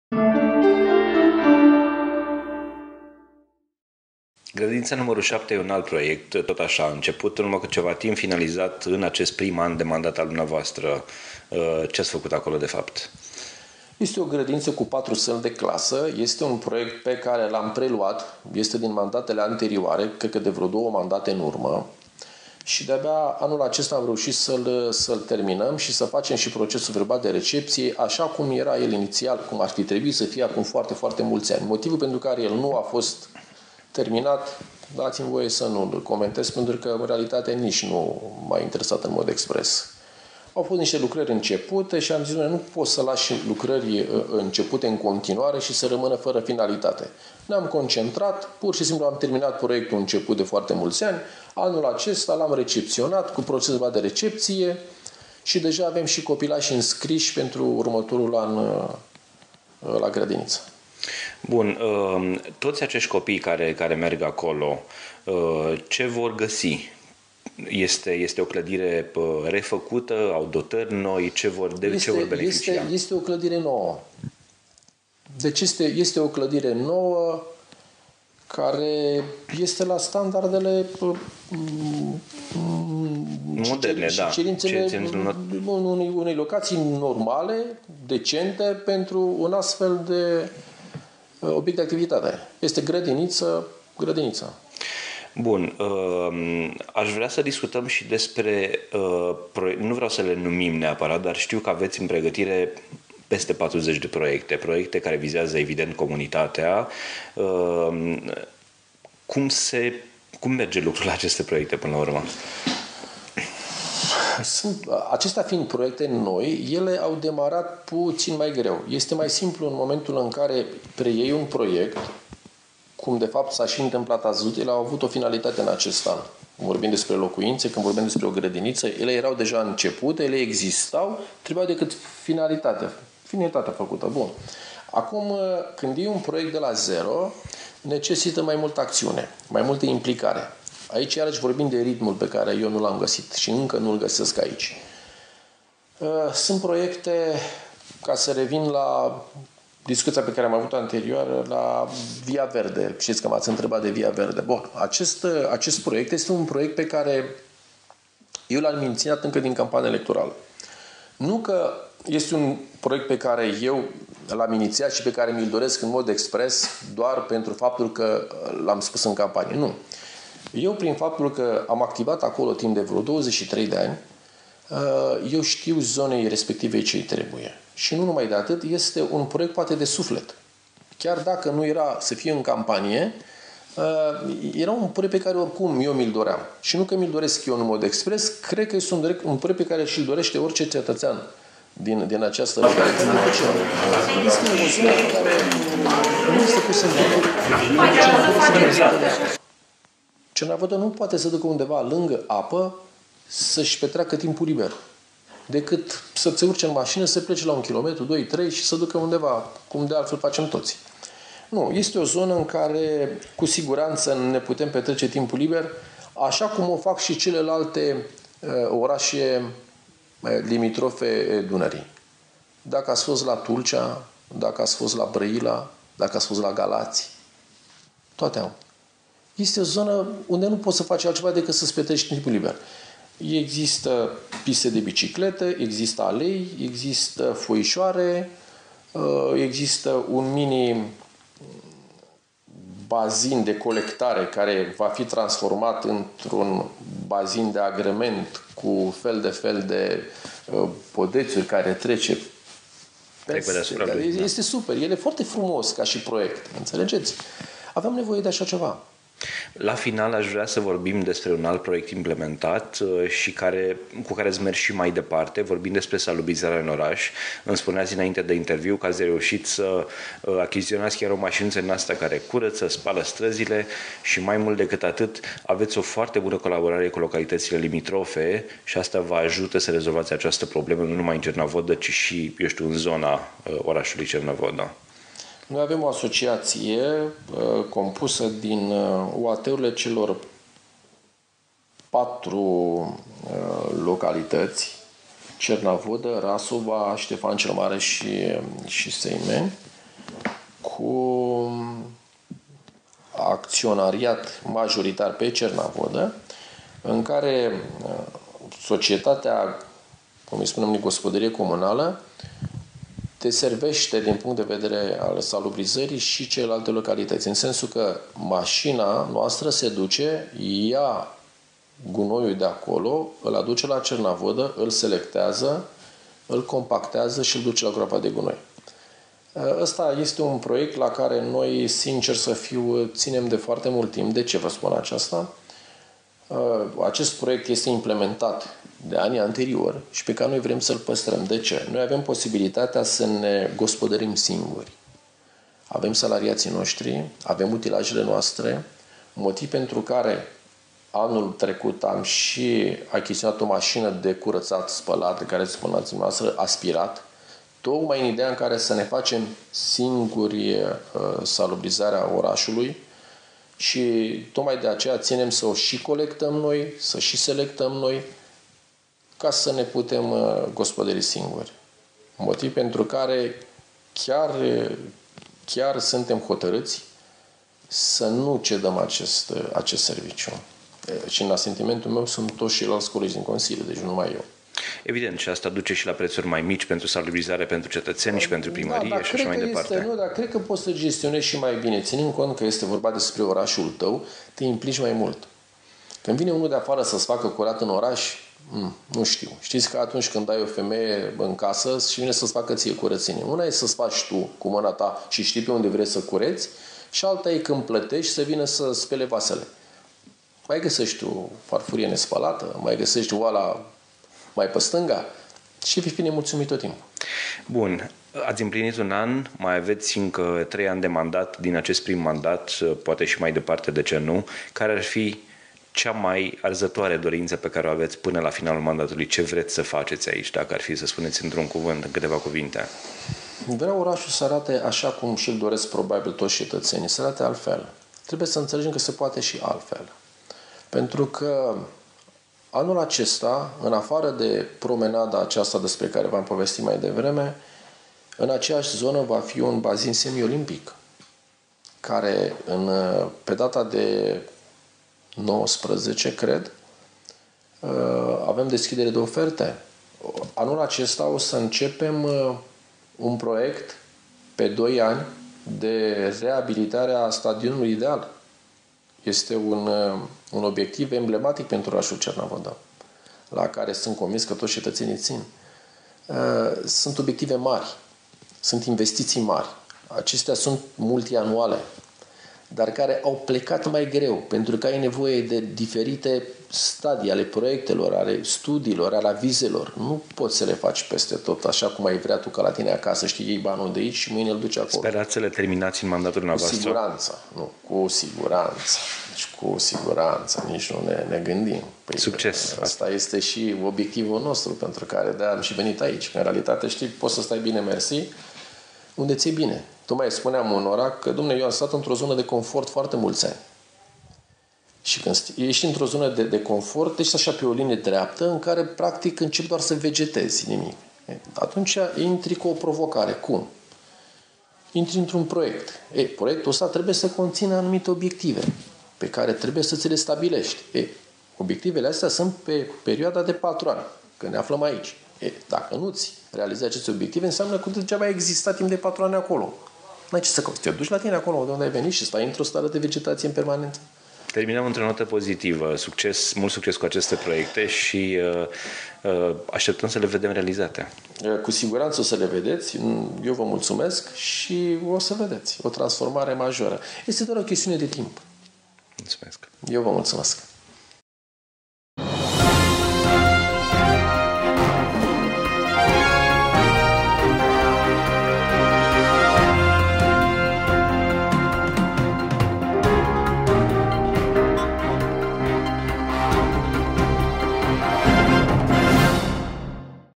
ctnews.ro | Primarul Liviu Negoita despre proiectele finalizate in primul an de mandat la Cernavoda
Primarul Liviu Negoita despre proiectele finalizate in primul an de mandat la Cernavoda